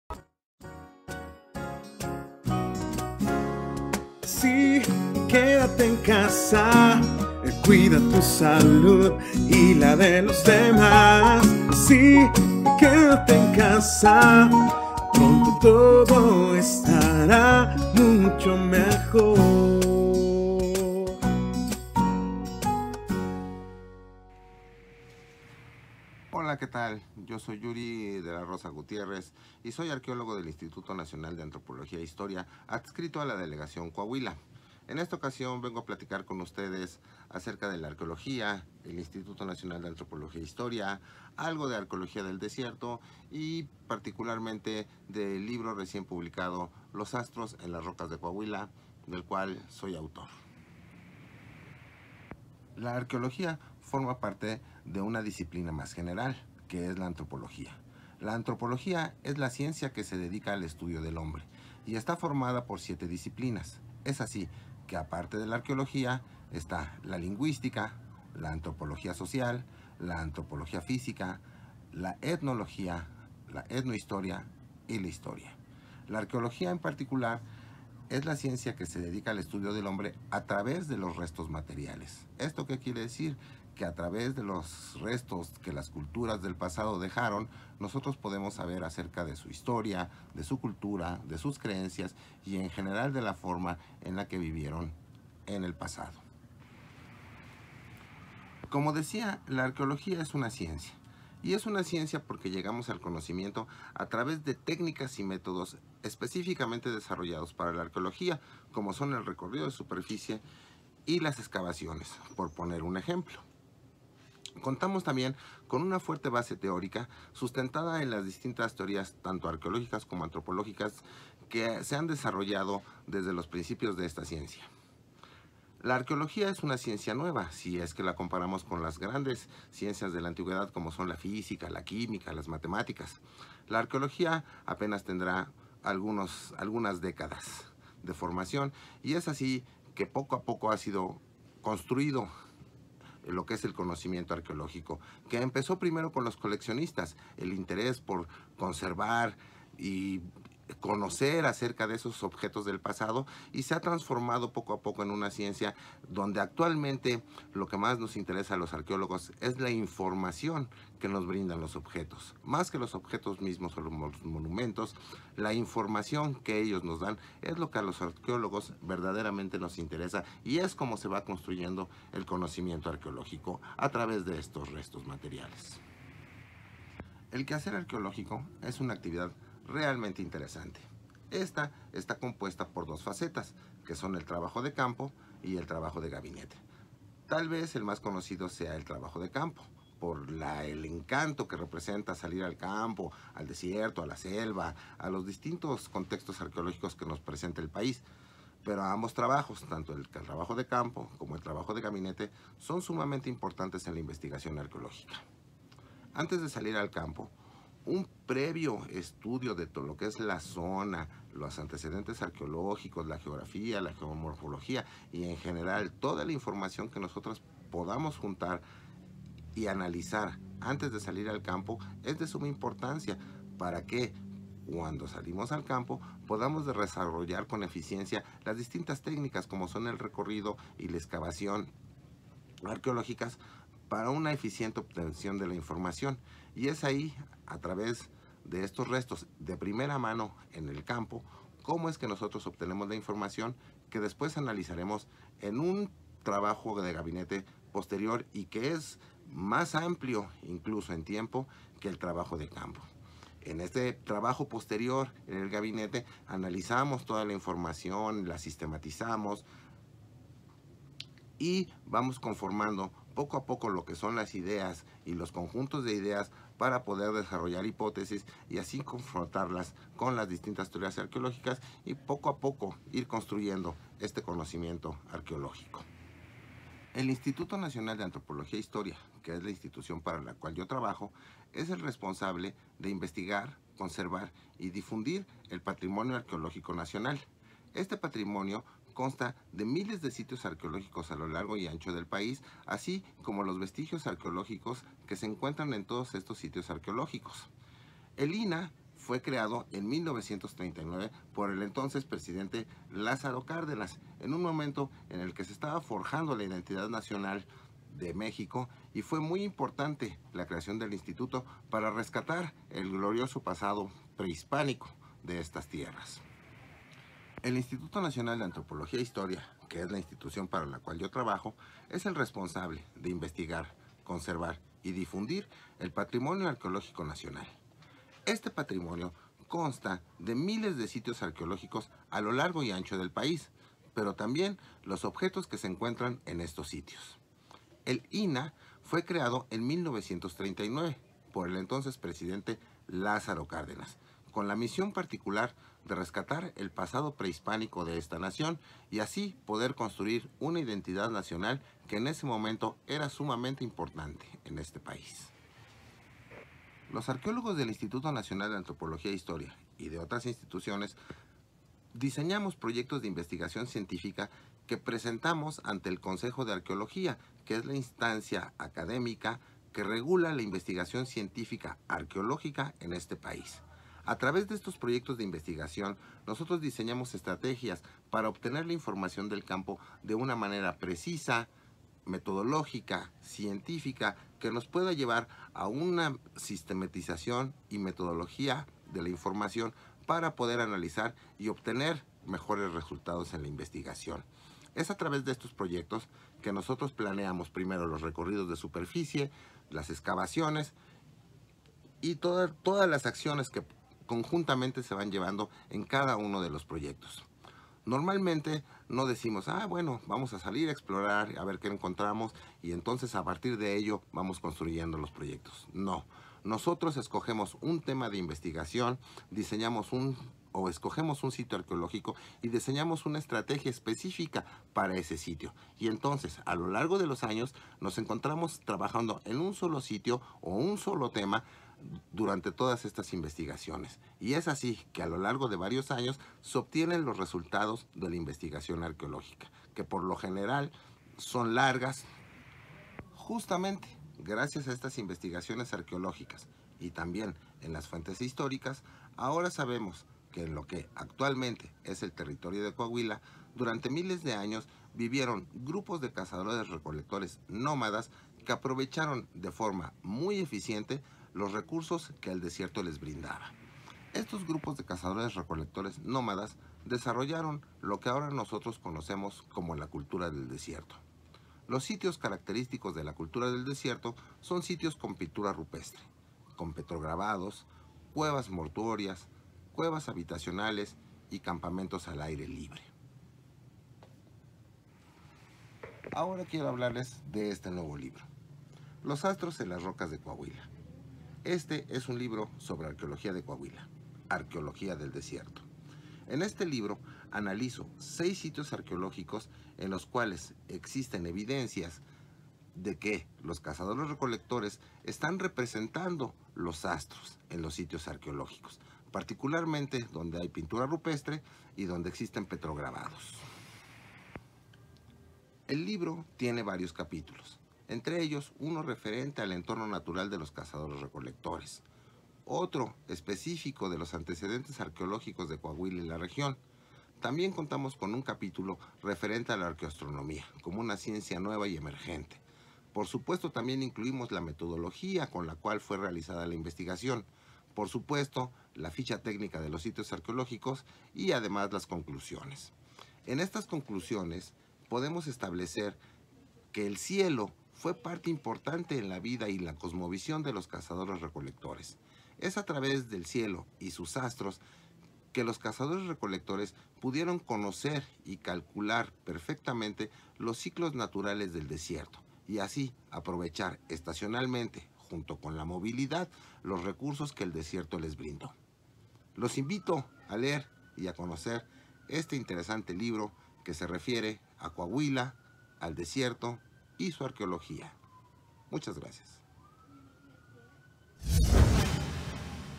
Arqueología de Coahuila y comentarios del libro Los astros en las rocas de Coahuila
Charla
Imparte Yuri de la Rosa Gutiérrez, arqueólogo del Centro INAH Coahuila, desde Cuatro Ciénegas